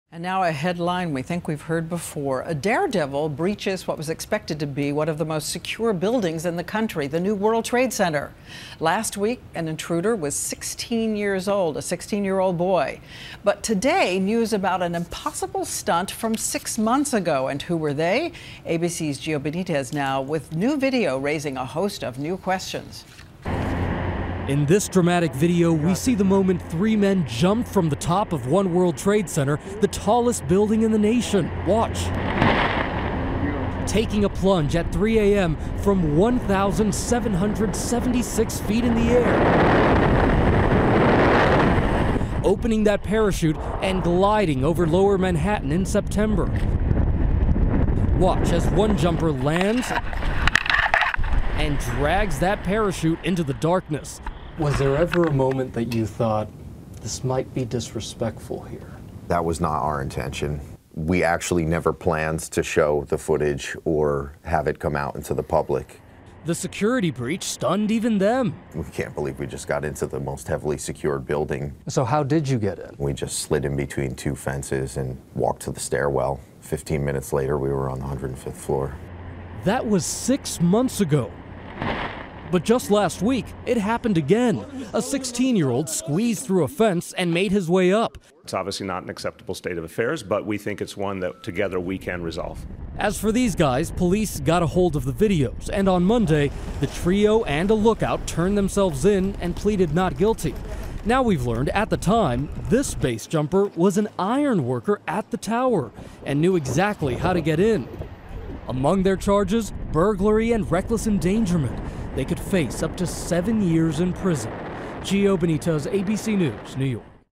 Caught on Tape: Base Jumping Off the World Trade Center
Intruders sneak up to 105th floor and parachute down.